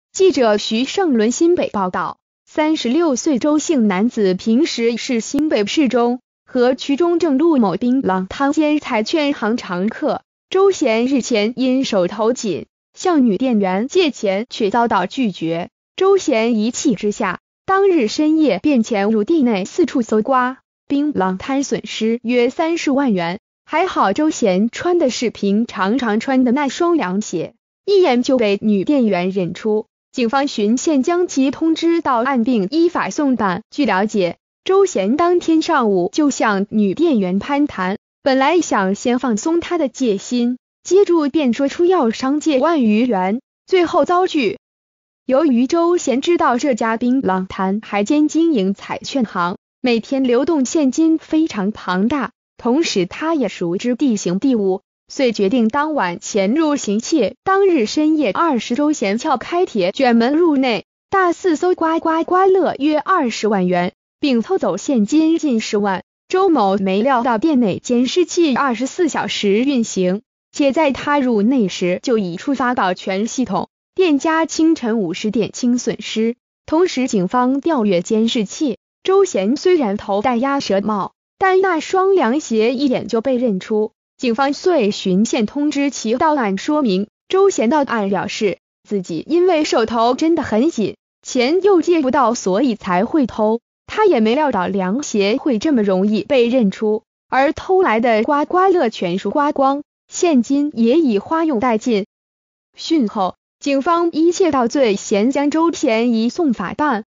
拒絕借錢遭常客偷30萬 彩券行店員認出涼鞋逮人
〔記者徐聖倫／新北報導〕36歲周姓男子平時是新北市中和區中正路某檳榔攤兼彩券行常客，周嫌日前因手頭緊，向女店員借錢卻遭到拒絕，周嫌一氣之下，當日深夜便潛入店內四處搜括，檳榔攤損失約30萬元，還好周嫌穿的是平常常穿的那雙涼鞋，一眼就被女店員認出，警方循線將其通知到案並依法送辦。

發表評論評論如下。